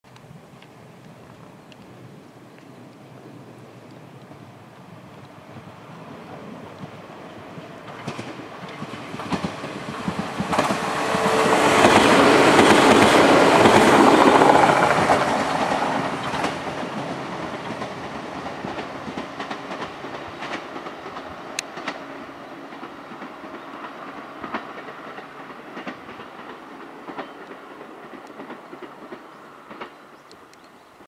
御坊⇔紀伊田辺の普通｜113系2000番台
JR西日本、きのくに線。紀伊田辺→御坊の普通として走るワンマン仕様の113系2000番台。和歌山県日高郡印南町、稲原‐和佐。

JR-West 113-2000 series. Local. Kinokuni Line.